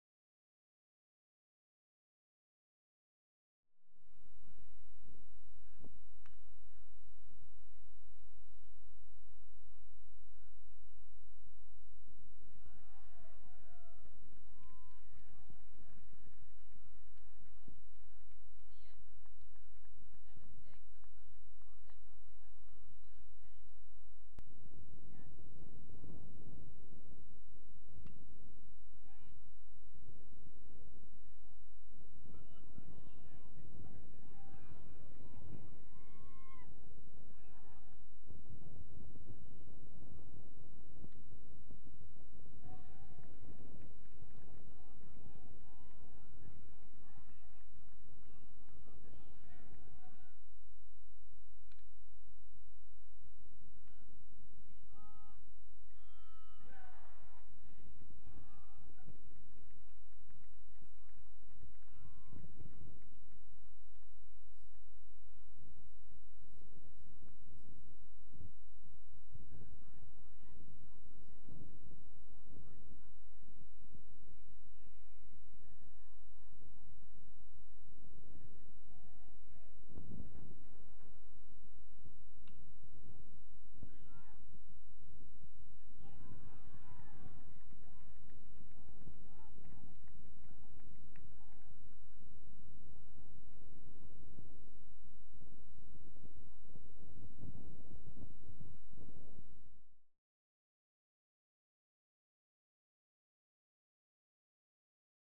BB Highlights v. Houghton 04-29-17
Highlights from the baseball team's 14-11 win v. Houghton on Senior Day on April 29, 2017.